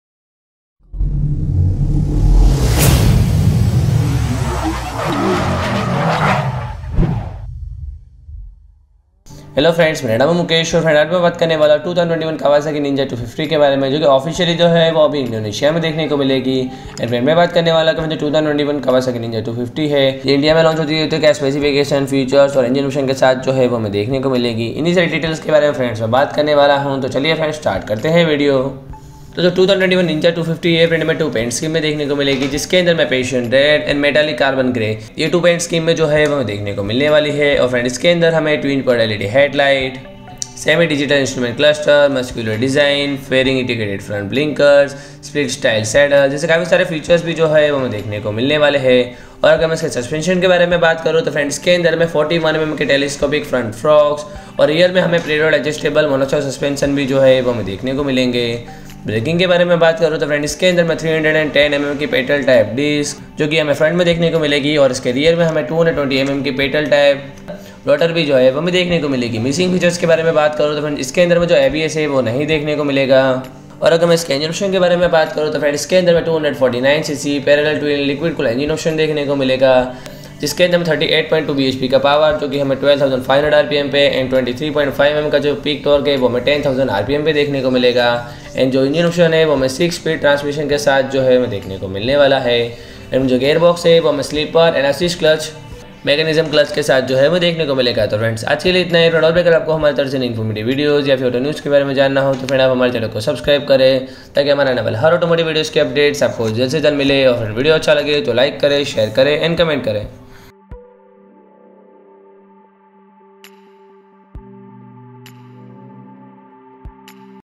2021 Kawasaki Ninja 250 Launched | India Launch Soon | All Changes, Spec, Features, Engine And More
Kawasaki has updated its Indonesian motorcycle portfolio with the launch of the 2021 Ninja 250. The motorcycle has been launched in two colour options – Passion Red and Metallic Carbon Grey – and it has been priced at 2021 Kawasaki Ninja 250 launched in Indonesia at Rp 64,200,000 (Rs 3.32 lakh). The aforementioned price is on-road, Jakarta. The feature list on the 2021 model continues to include a twin-pod LED headlight, a semi-digital instrument cluster, muscular design, fairing integrated front blinkers, and a split-style saddle. The suspension setup on the motorcycle comprises of 41mm telescopic front forks and a preload-adjustable rear mono-shock. The braking setup includes a 310mm petal-type disc at the front and a 220mm petal-type rotor at the back. The safety net is provided by ABS. Mechanical specifications include a 249cc, parallel-twin, liquid-cooled engine. Linked to a six-speed transmission, the motor makes 38.2bhp at 12,500rpm and 23.5Nm of peak torque at 10,000rpm. The gearbox benefits from a slipper and assist clutch mechanism.